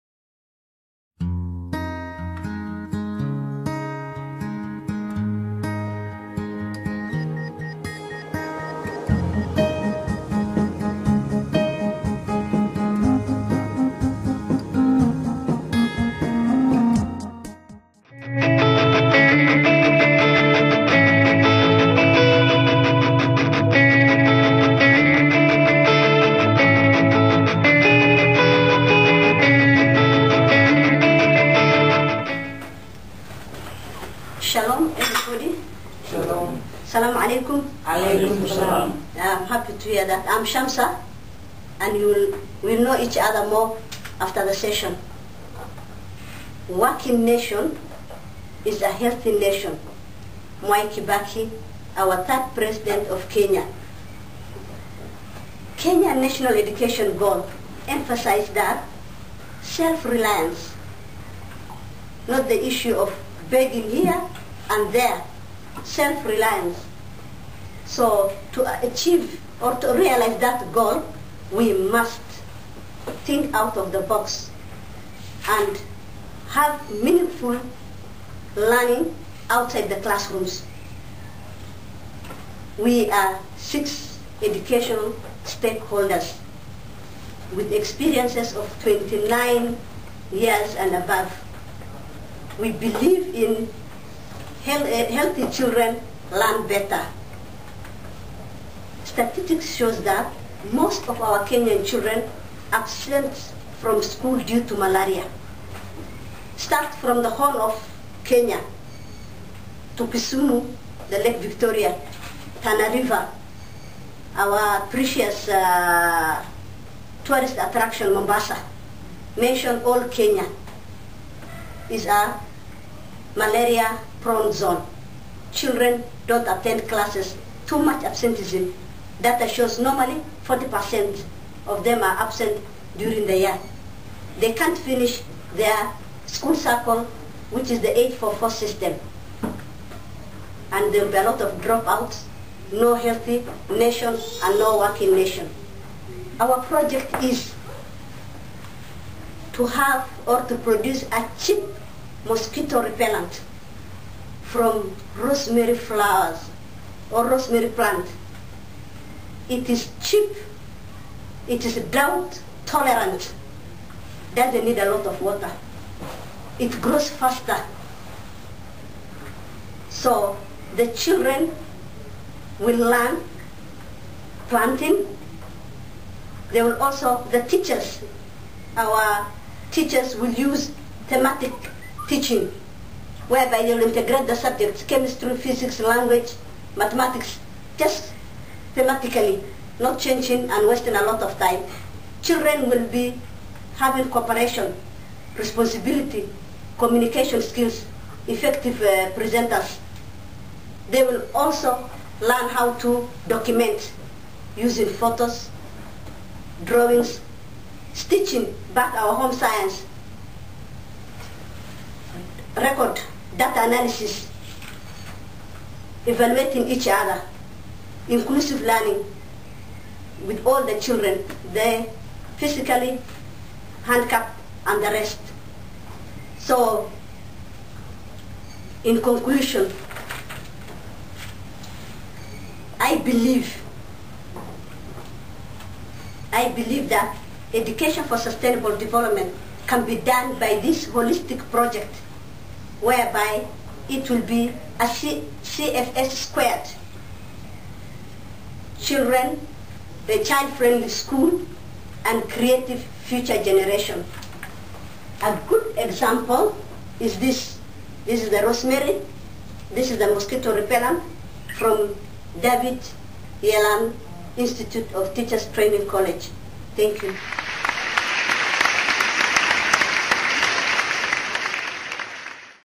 Educational Initiative: "School Mosquito Free Zone"
"Many learners miss significant number of lessons due to malaria caused by mosquitoes.  The records from the school attendance register shows that cumulative absenteeism days translate to  40%.This has led to poor performance and dependent people who are not able to make meaningful life.
Trough planting mosquito repellent plants we will be able to increase lesson attendance and multiplications effect..."
Training Course: Education for Sustainable Development - KENYA 2014